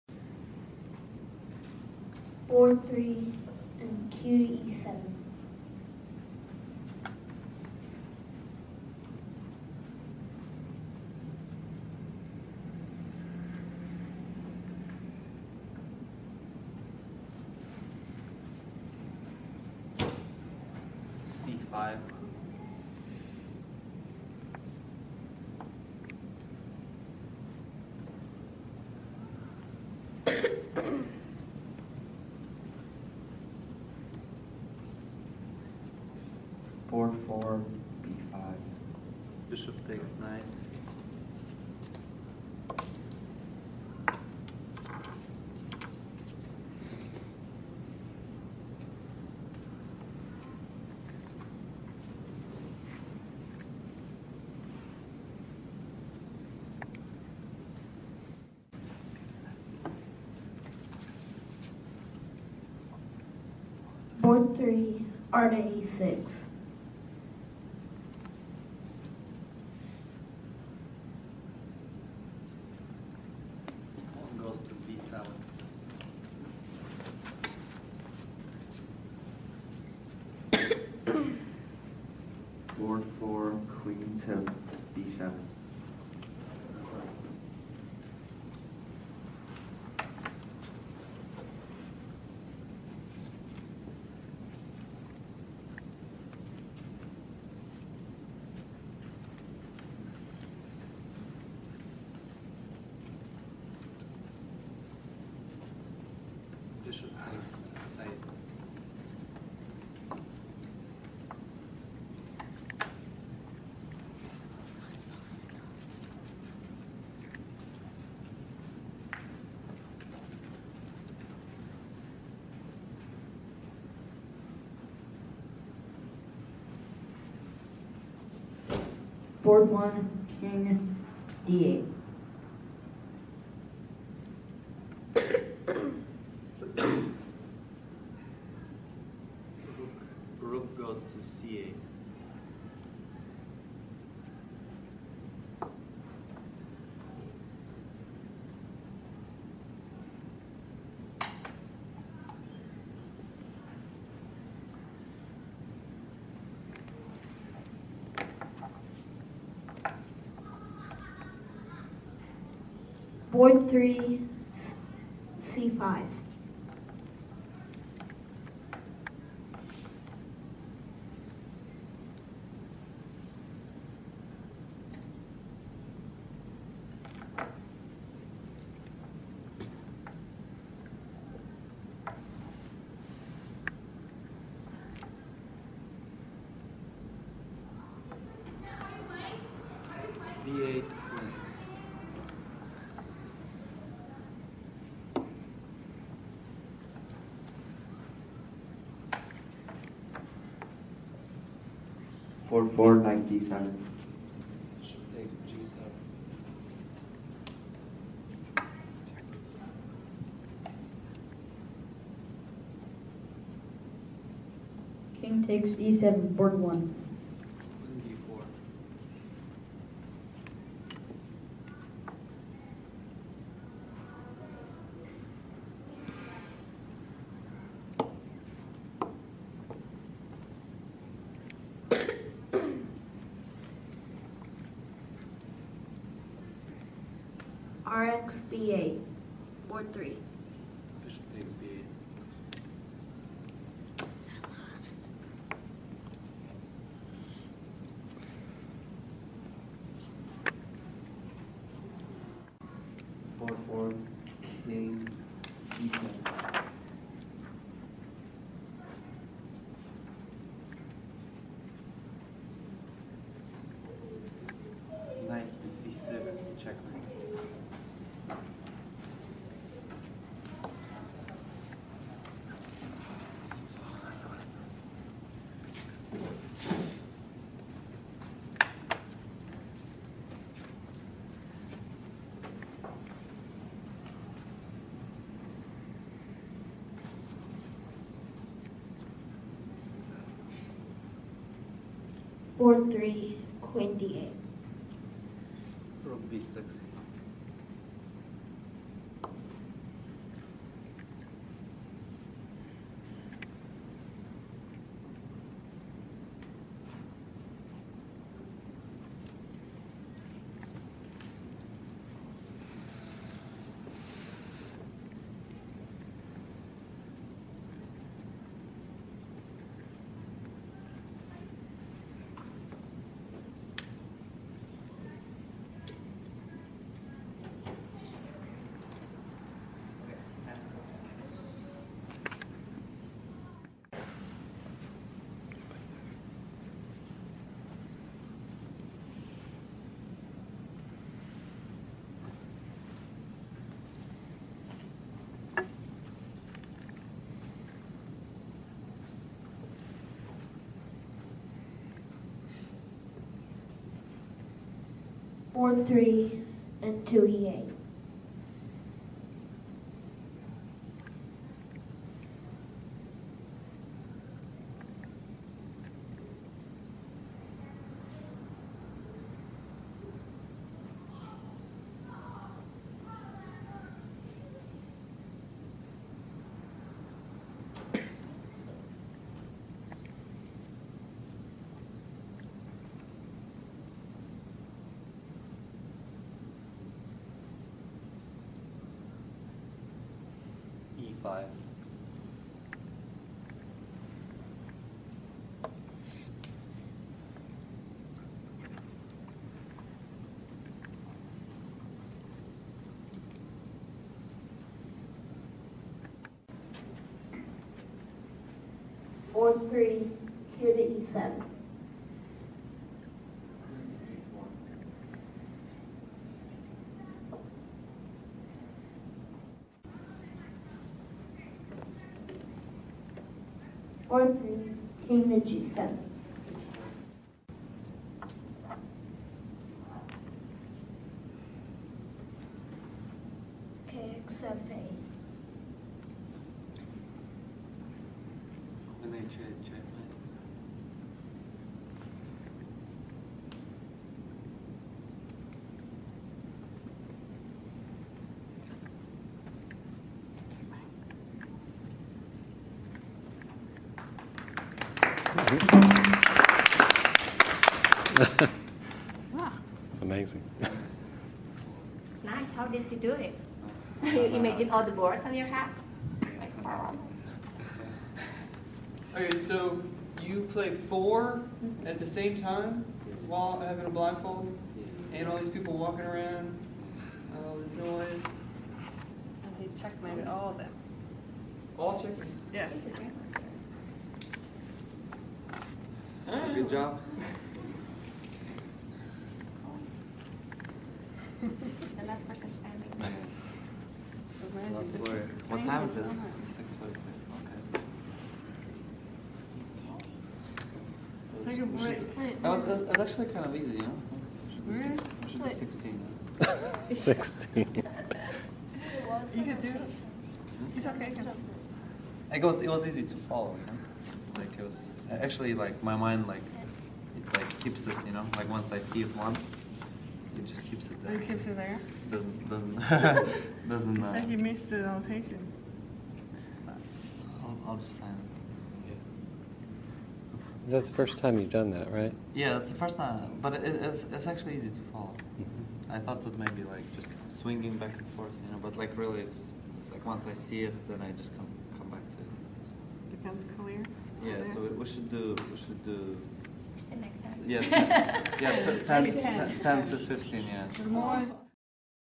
GM Timur Gareev Blindfold Simultaneous Chess Exhibition Part 2 April 23, 2012
GM Timur Gareev performs blindfold simultaneous chess exhibition with 4 boards at the Laura Bush Library. Hosted by Checkmate Kids Chess Club in Austin TX. GM Timur Gareev won all 4 games. Julia Hall (seen in the blue shirt) makes the moves for Timur.